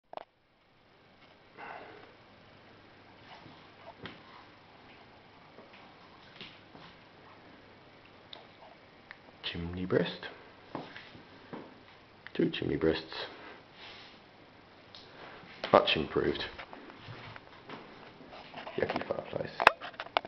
After the ripping
After ripping most of the tongue and groove cladding out of the kitchen - so much better & lighter now - feels like a nice room to be in.